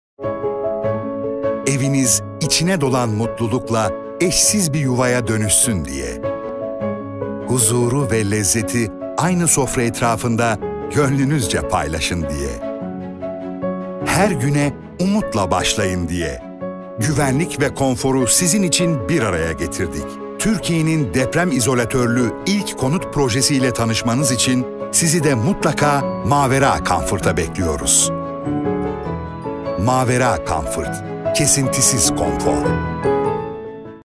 Alp Özbek - Mavera Comfort
Oyuncumuz Alp Özbek'in rol aldığı Mavera Comfort reklamı